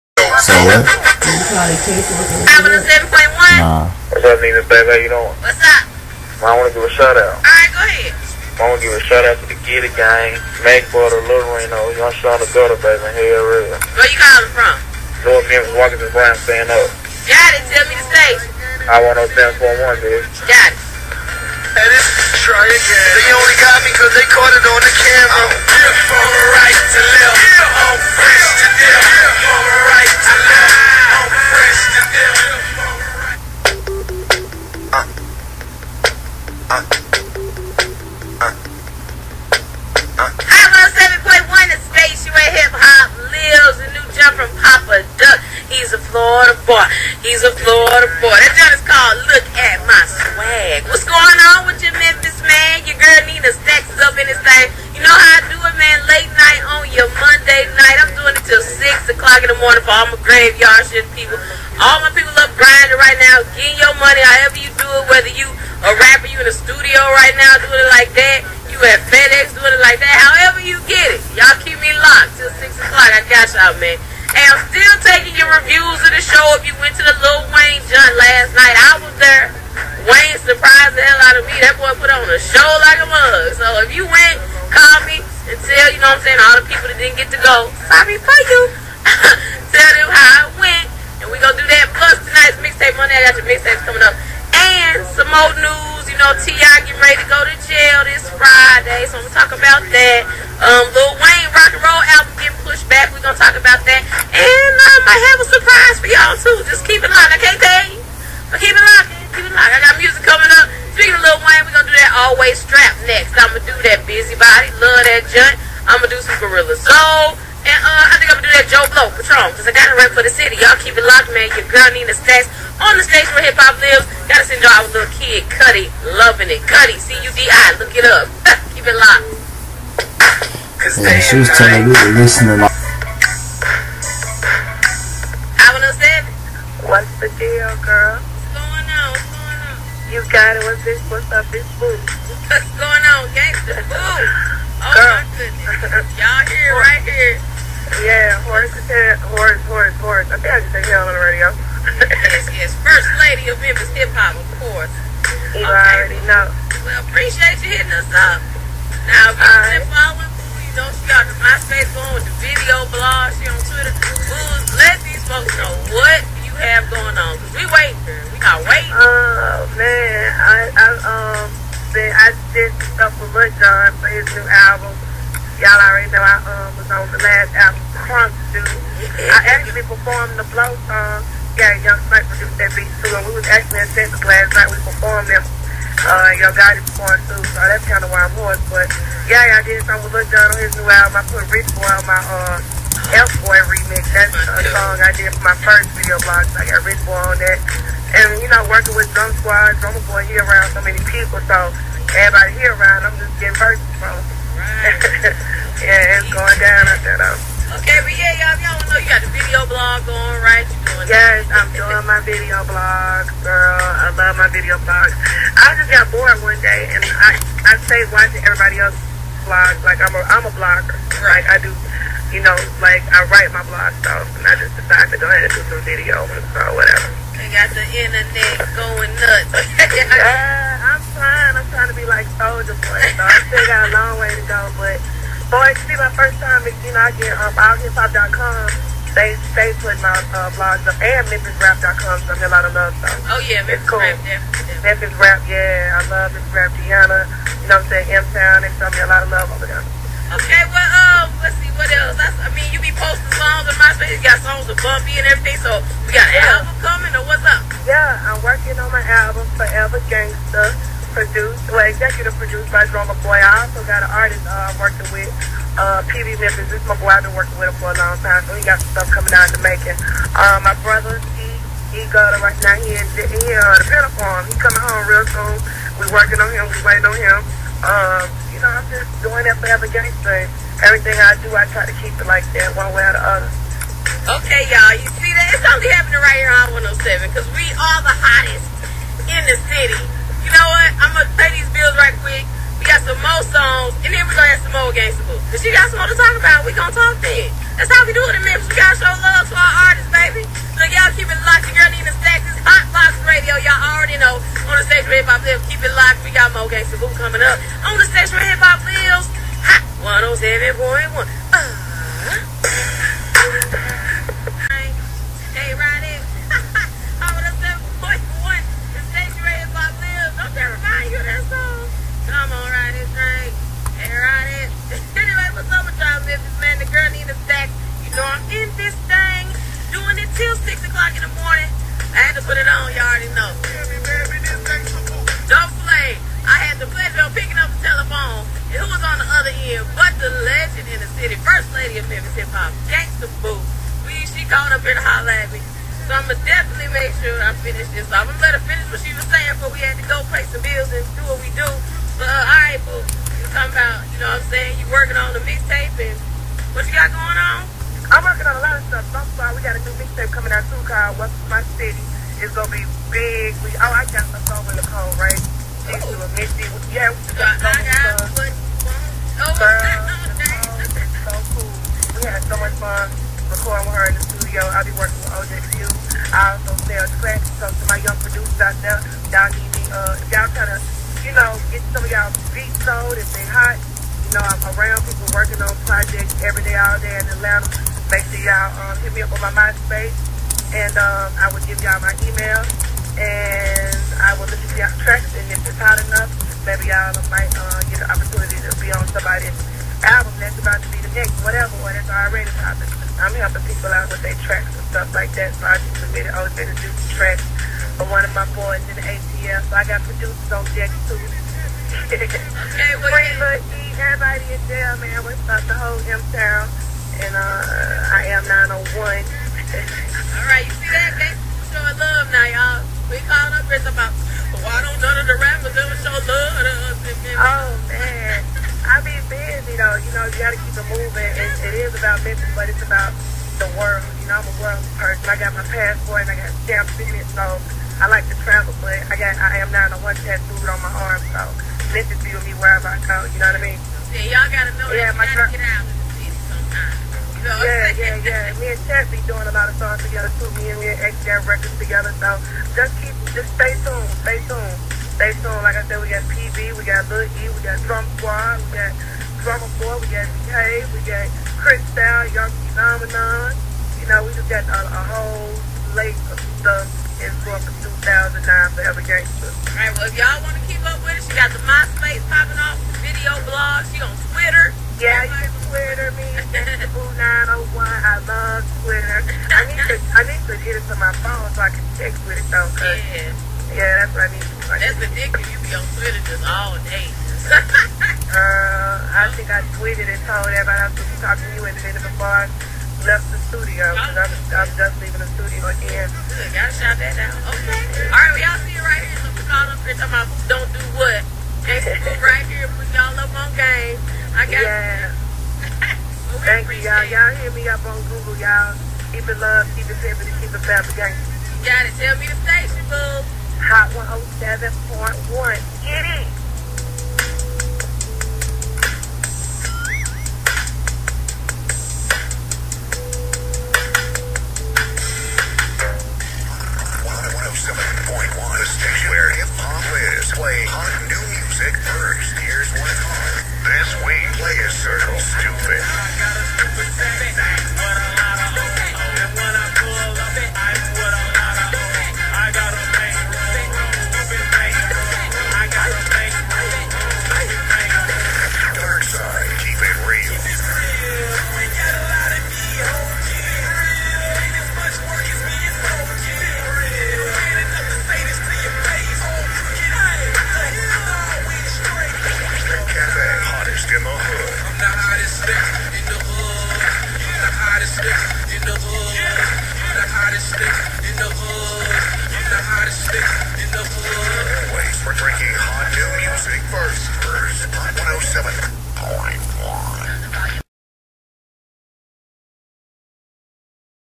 GANGSTA BOO INTERVIEW
Gangsta Boo Interview with Dj Nina Stakz on hot 107.1